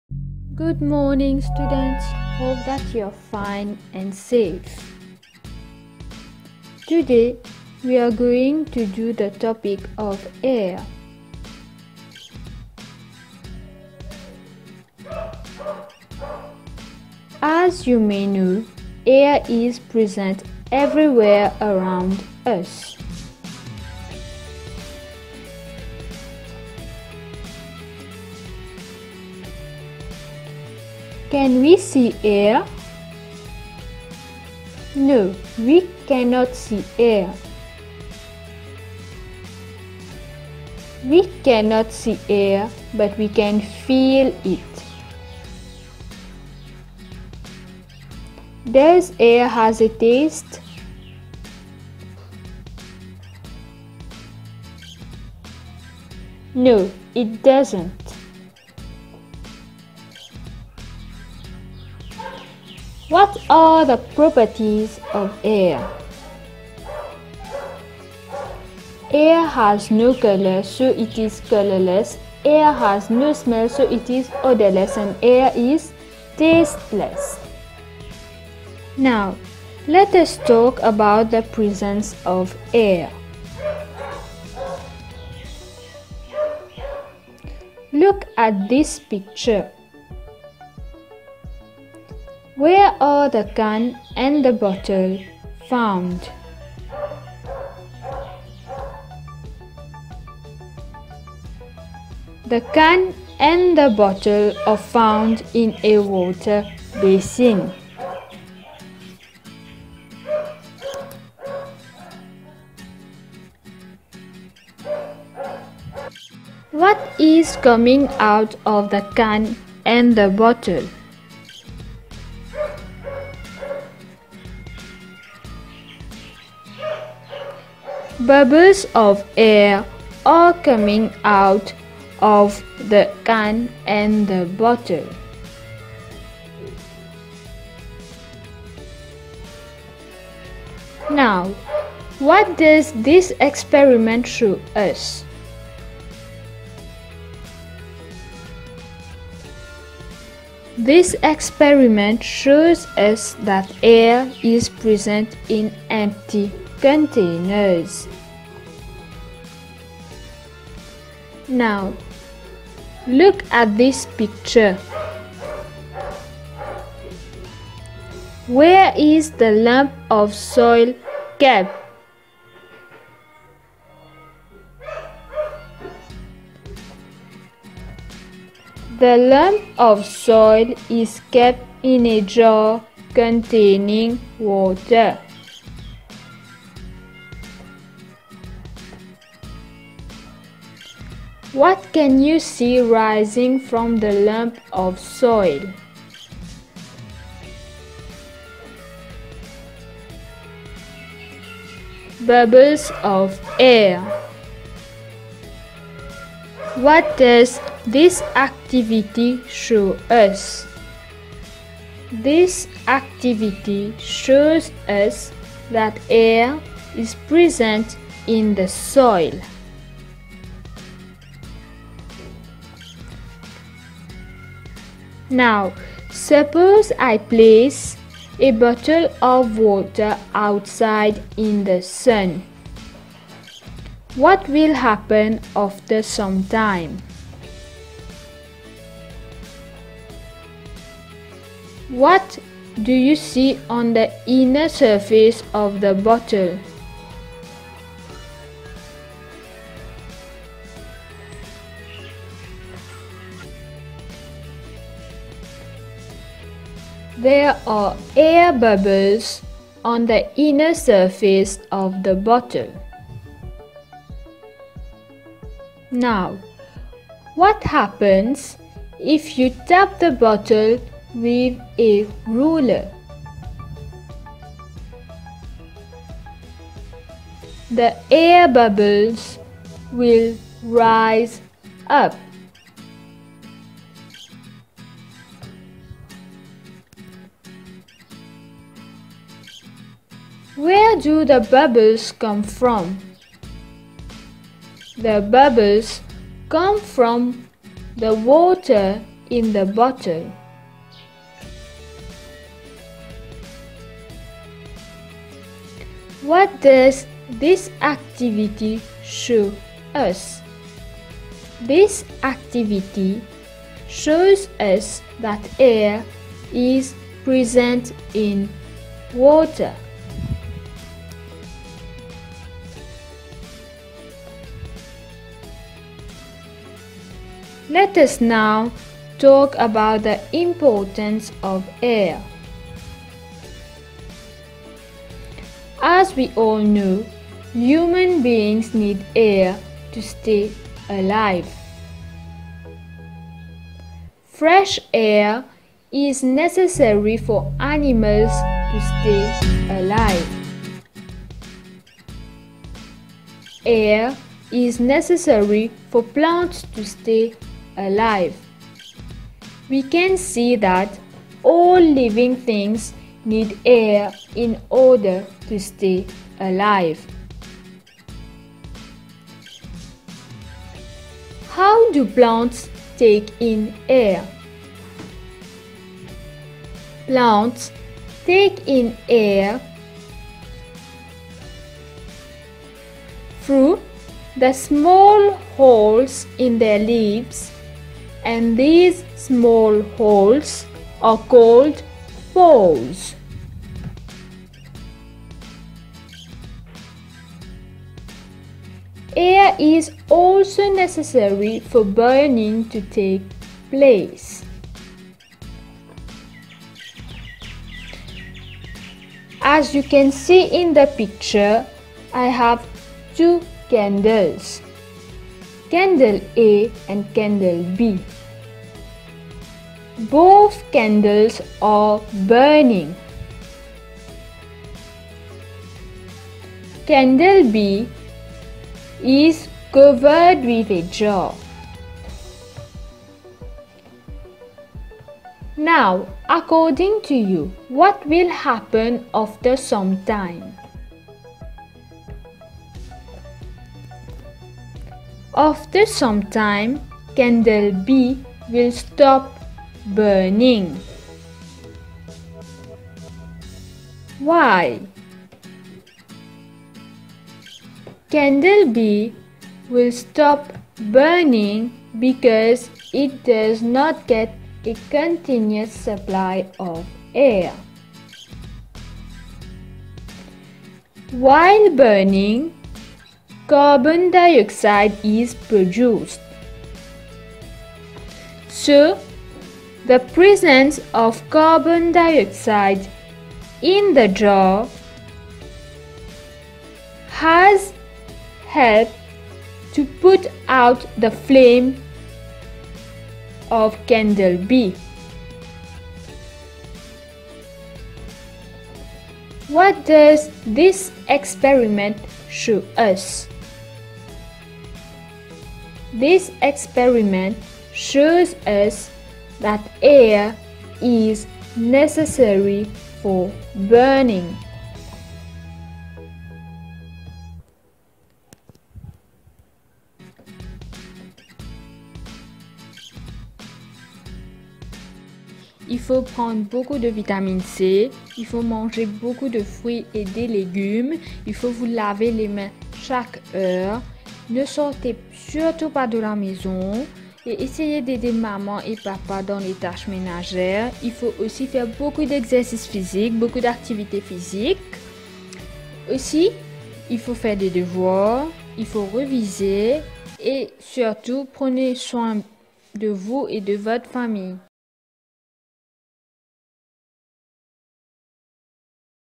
BCC School Grade 4- Science Topic Air (Thursday 16.04.2020)
Our kids are NOT lagged behind due to lockdown. The school management and teaching staff members are working hard from home for the betterment of our students. Here is a video for Grade 4 - Science covering the topic Air. Enjoy learning from home with BCC School. Stay Safe.